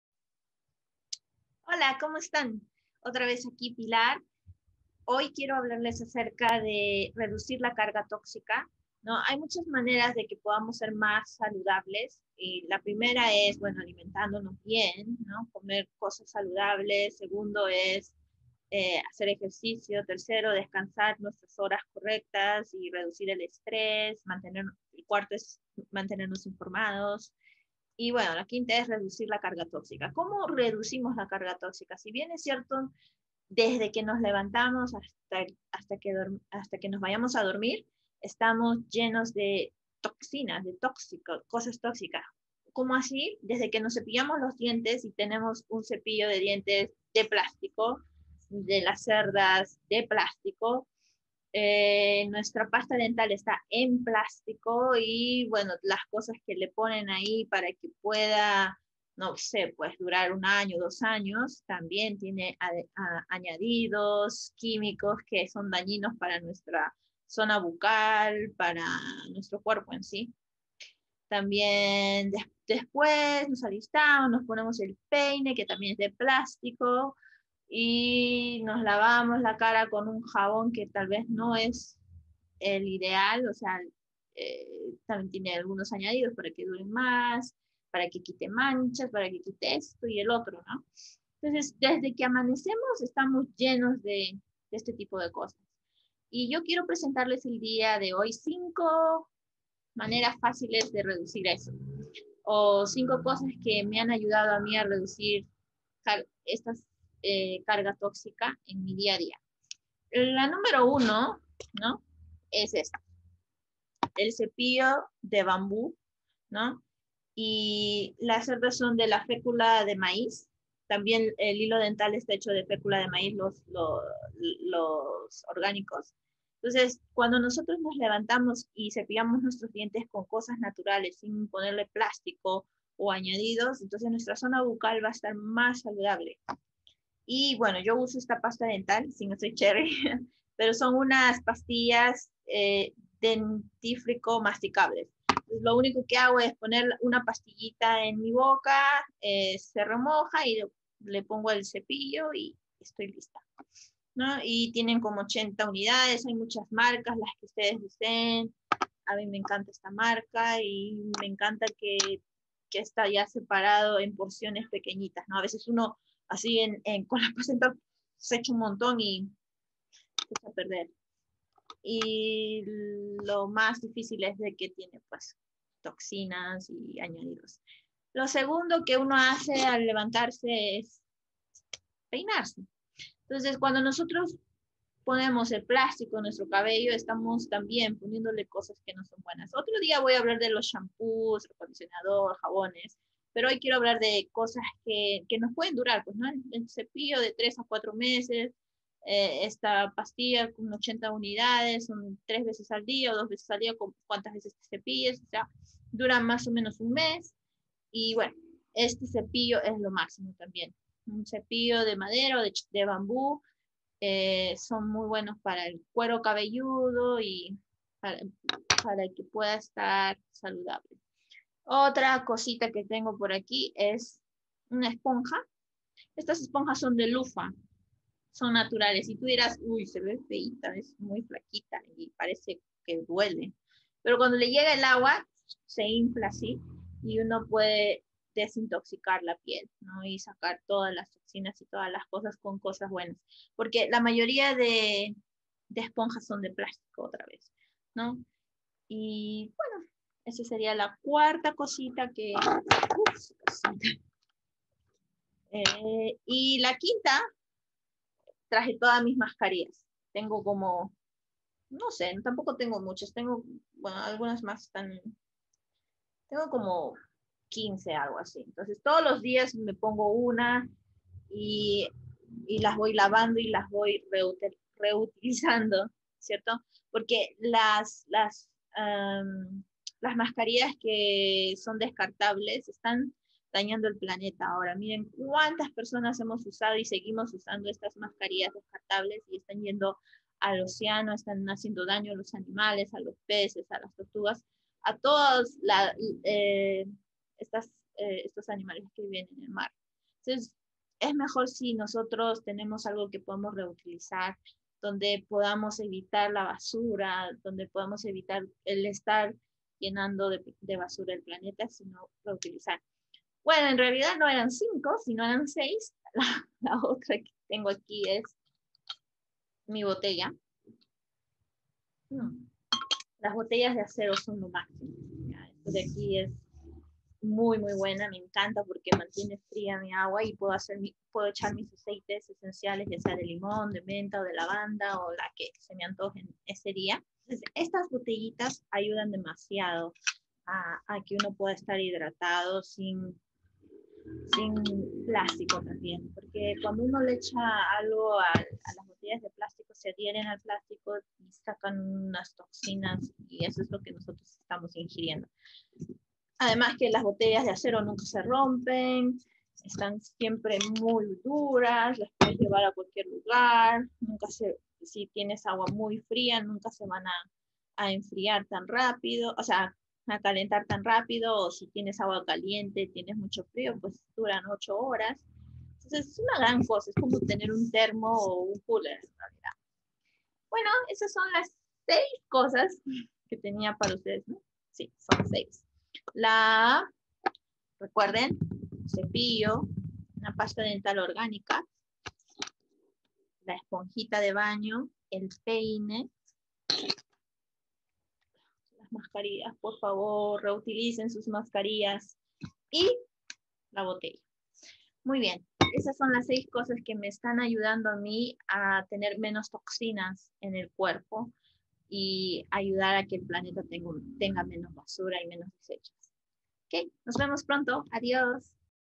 6 formas básicas de reducir la carga tóxica en nuestro cuerpo
Desde que nos levantamos hasta que nos vamos a dormir, nuestro cuerpo recibe gases tóxicos. Desde los que comemos, olemos, tocamos, escuchamos o vemos bien libera o aumenta la toxicidad en nuestro organismo. Por tanto, es importante tomar medidas de salud y bienestar urgentes.